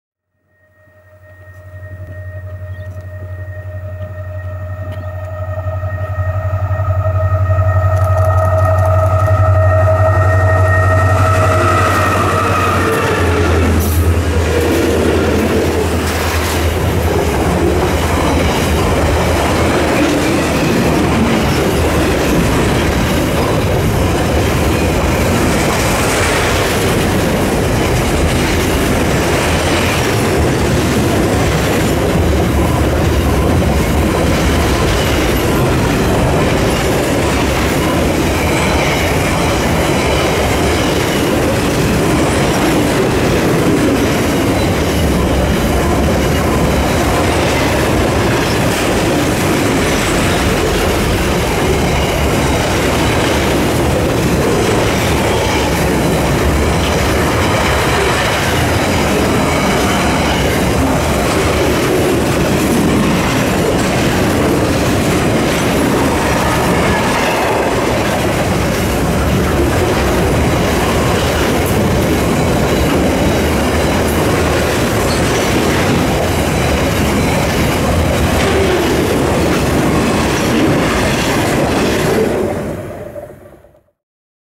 GWA 7AD1 are seen nearing Bolivar
7AD1 is seen nearing Bolivar with GWU007, ALF21 & CLF5 up the front on their way to Darwin.

Saturday 14th of September 2013